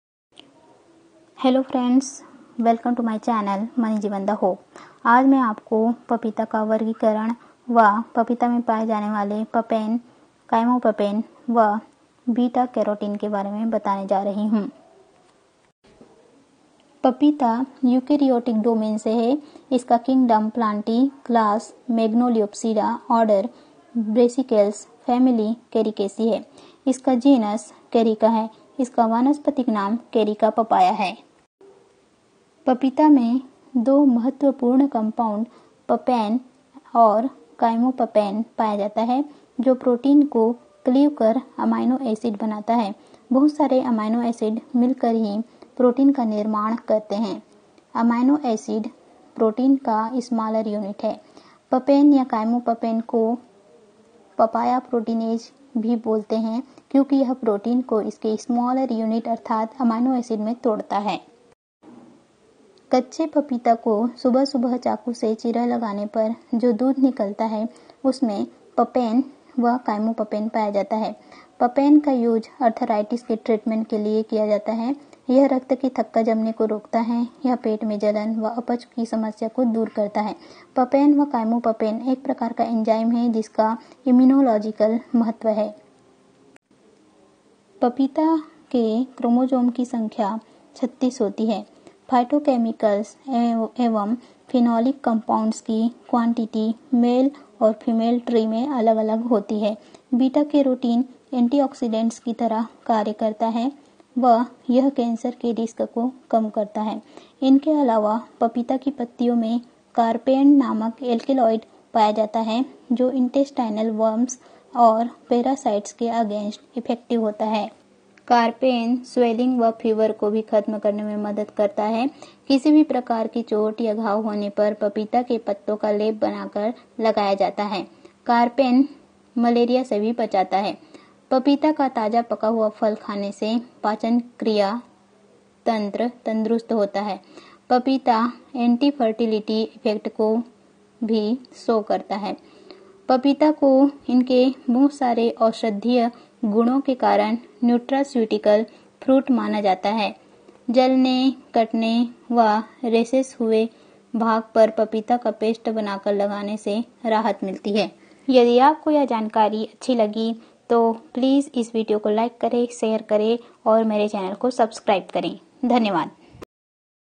Pytochemicals in Papaya, papita me paye jane wale phytochemicals
in this video i tell about Phytochemicals in Papaya and classification of papaya.

i am very very thankful to you.